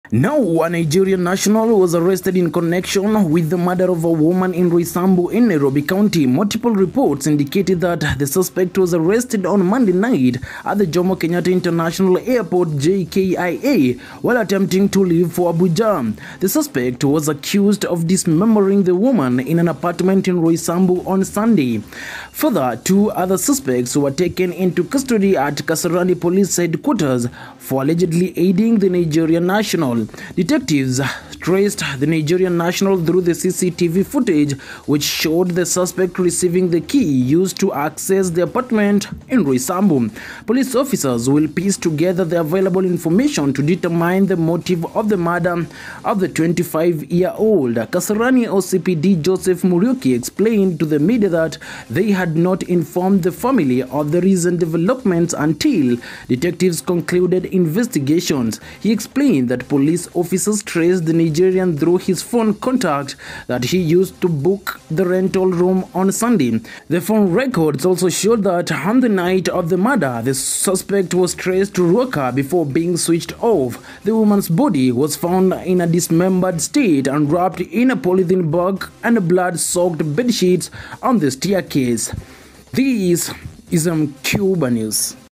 Nigerian man arrested over Roysambu Airbnb Murd€r while trying to flee to Abuja
A Nigeria national is arrested in connection to the Roysambu Thika Road Airbnb saga

CS Moses Kuria Hits Back To Dp Gachagua